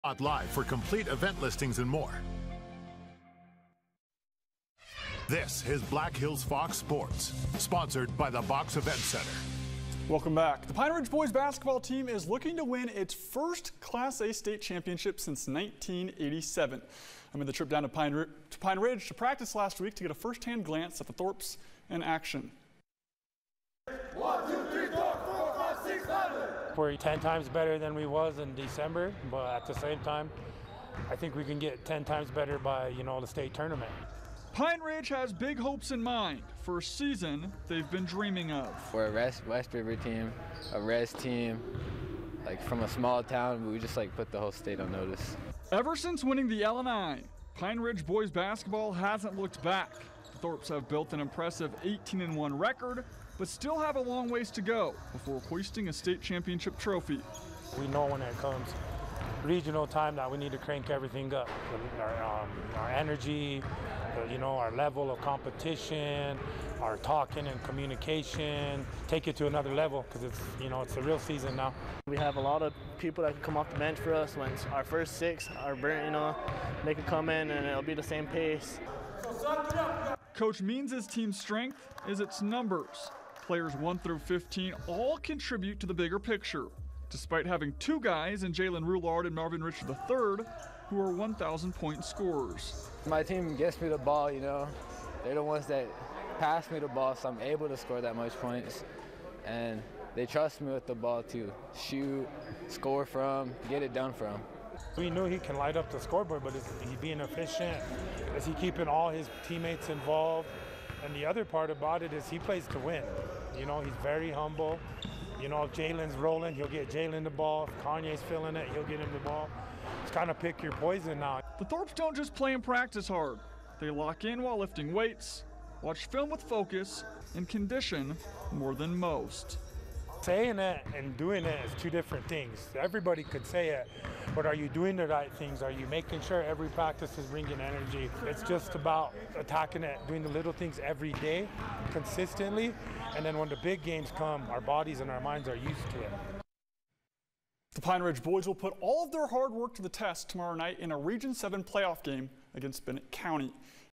‘It’s the real season now’: Pine Ridge boys are postseason ready
The Pine Ridge boy's basketball team is chasing its first Class A state championship since 1987.